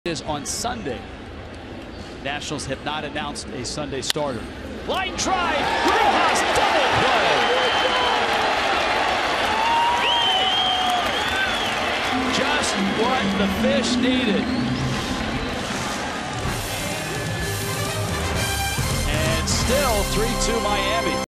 MIL@MIA: Rojas snags liner, steps on first for DP
Miguel Rojas snags a liner off the bat of Aaron Hill, then steps on first to double off Jonathan Lucroy for the unassisted double play